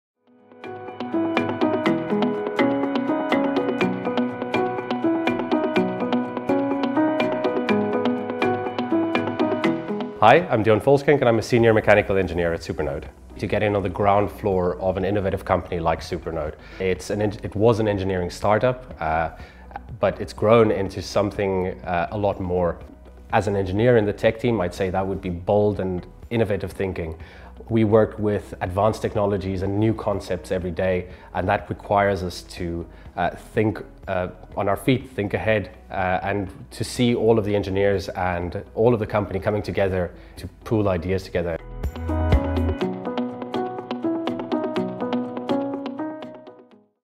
SuperNode Employee Testimonial Deon Volschenk, Senior Mechanical Engineer
Employee Testimonial from Deon Volschenk, Senior Mechanical Engineer at SuperNode Ltd.